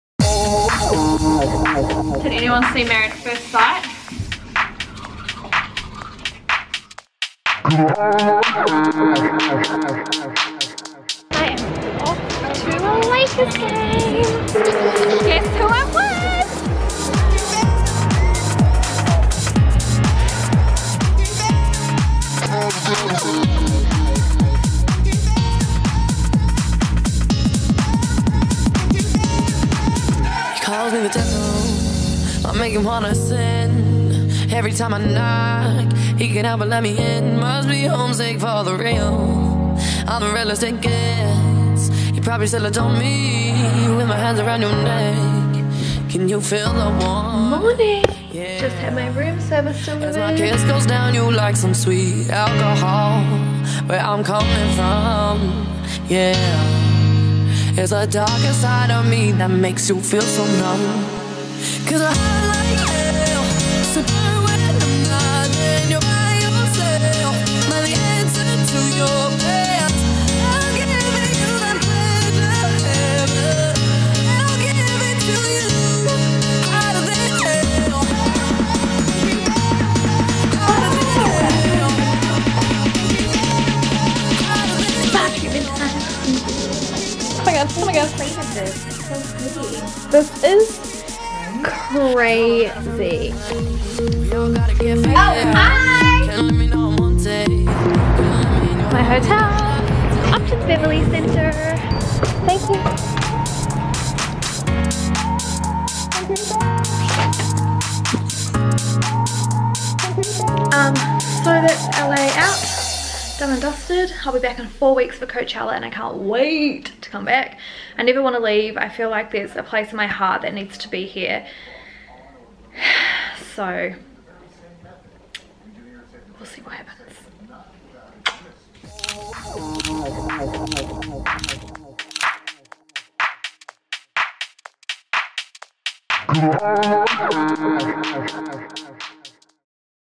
LA BABY | Travel Vlog PART 3
LA was a short trip but never the less I love it.

Some places I suggest checking out in LA are

- CATCH LA
- Delilahs
- Melrose Ave
- Venice Beach
- Santa Monica
- SUR
- Hollywood Sign
- Runyon Canyon
- Staples Center ( If you love Basketball )
- Millennium Dance Complex / The Edge ( If you are a dancer )
- EP and LP

Get a Sim card its cheap! and Uber everywhere. Easy Peasy

I hope you loved my Vlog for USA!

SHARE WITH A FRIEND WHO IS PLANNING A TRIP TO USA

Much Love xo